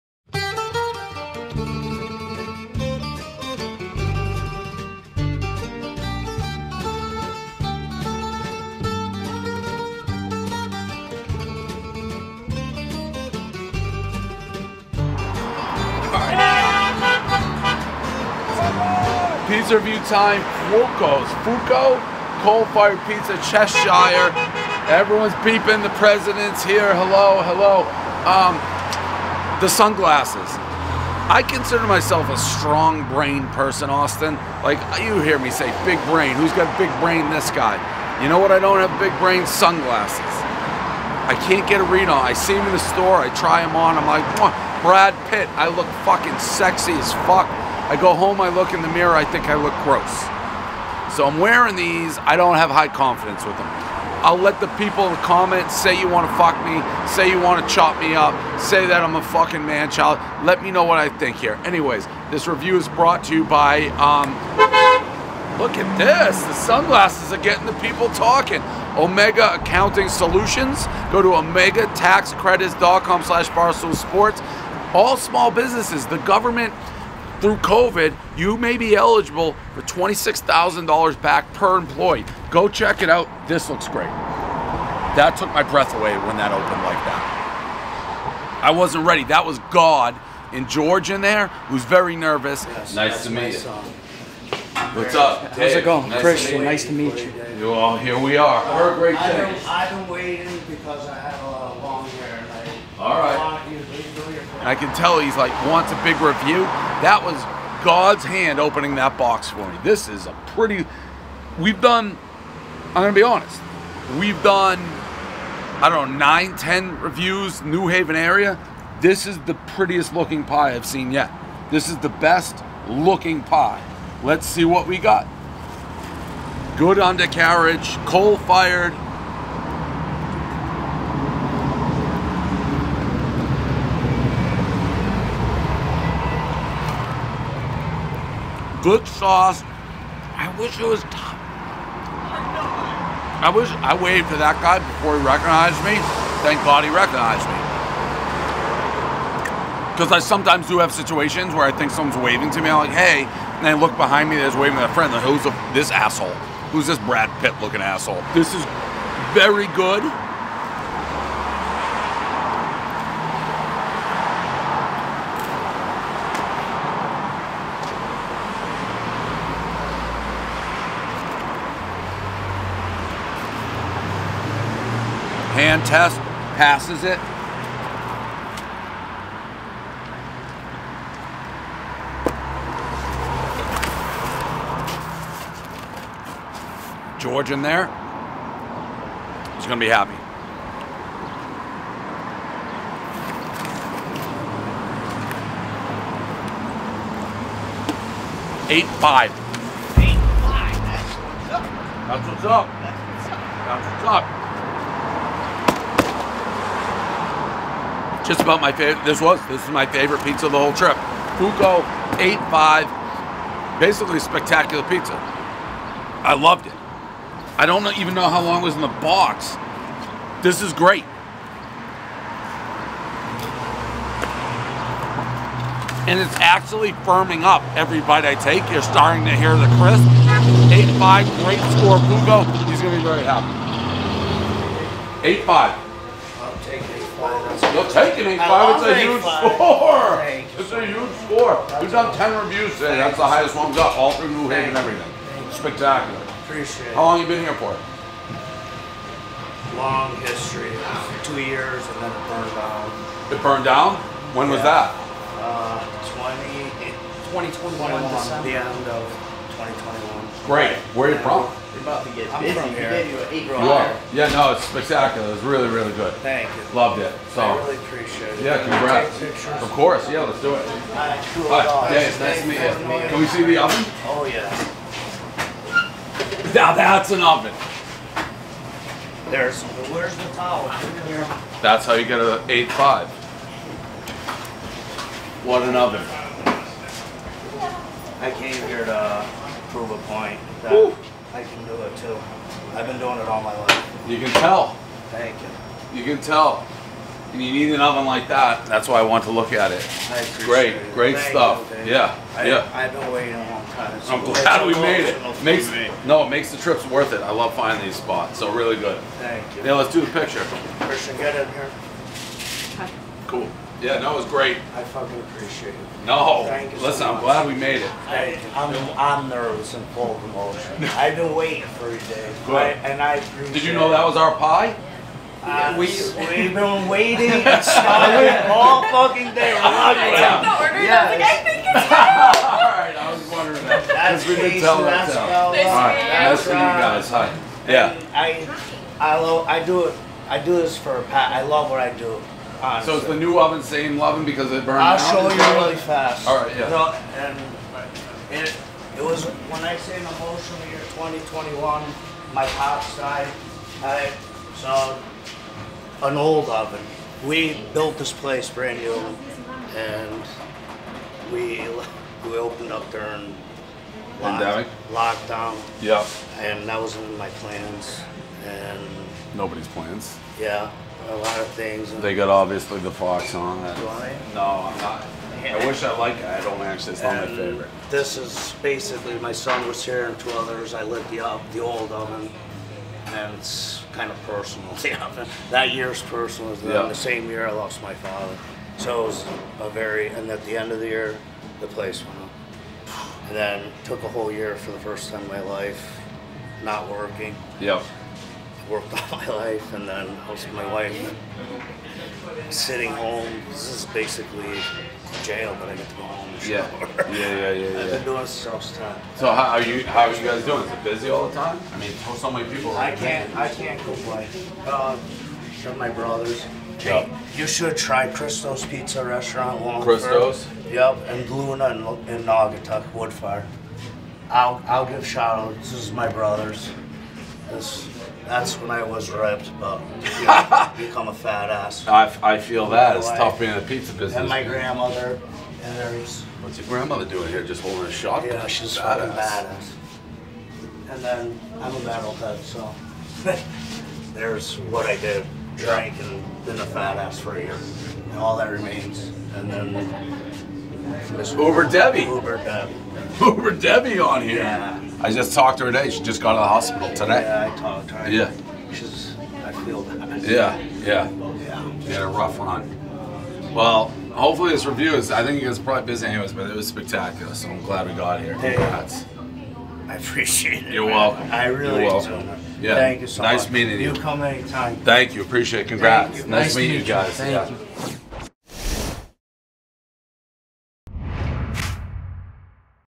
Barstool Pizza Review - Fuoco Apizza (Cheshire, CT) presented by Omega Accounting Solutions
Dave stops by the heavily recommend Fuoco Apizza, a place that burned down in 2021 and is on the way back up.

NOW to receive a free consultation in less than 10 min